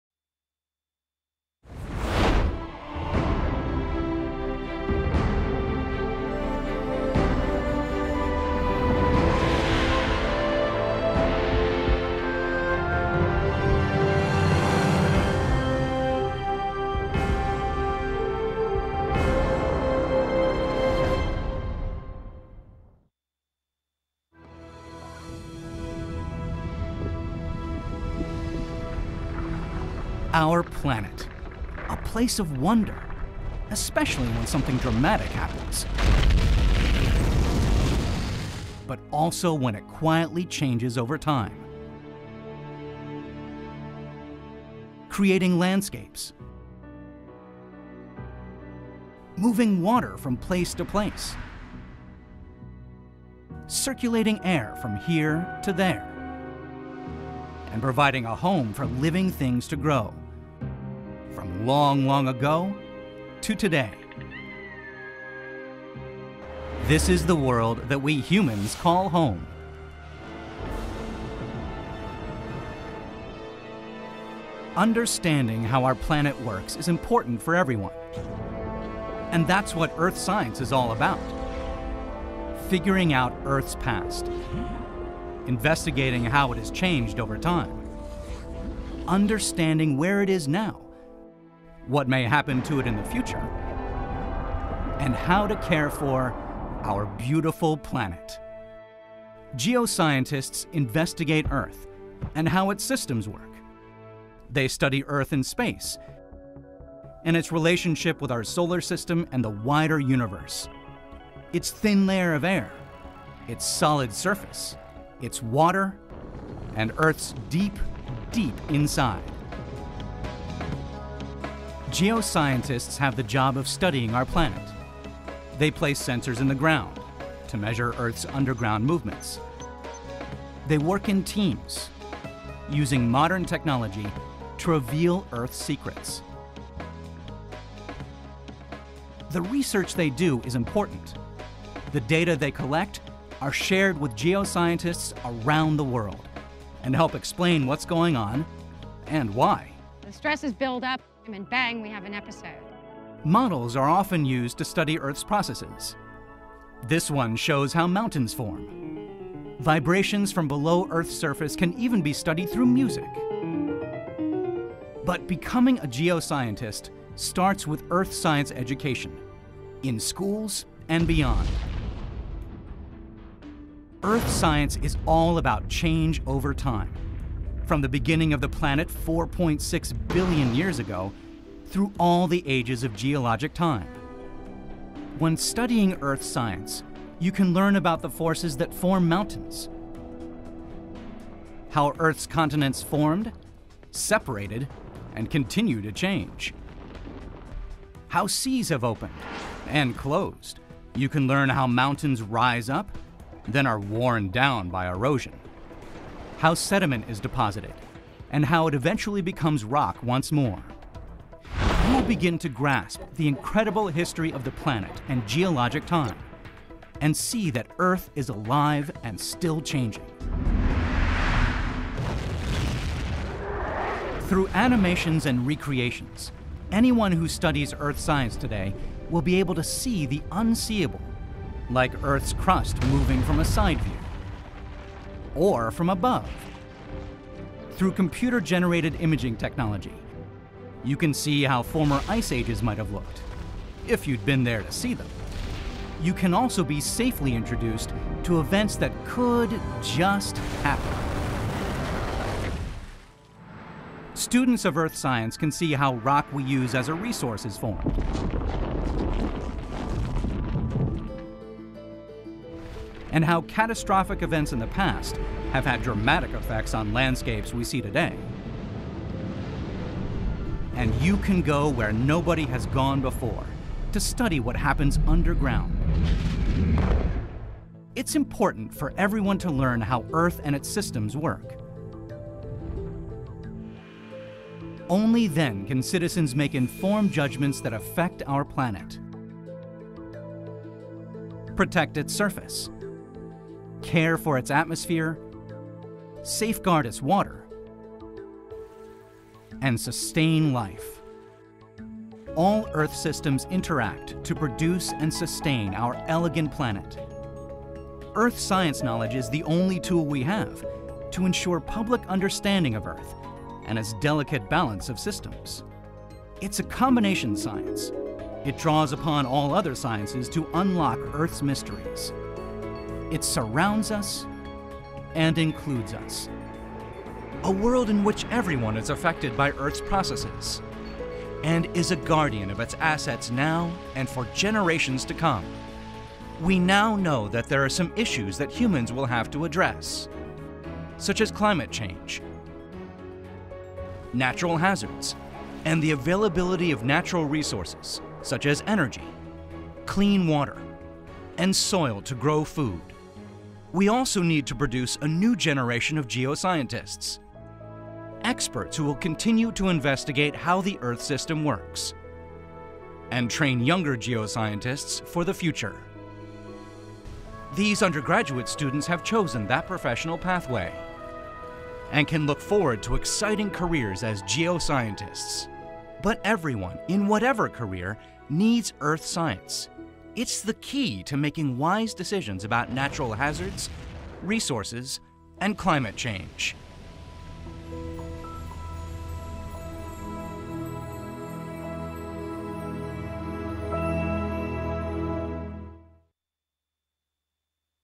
¿Por qué ciencias de la Tierra?
Las ciencias de la Tierra son muy importantes para entender y cuidar nuestro hogar. Video del American Geological Institute con subtítulos en español del Comité Mexicano para el Año Internacional del Planeta Tierra.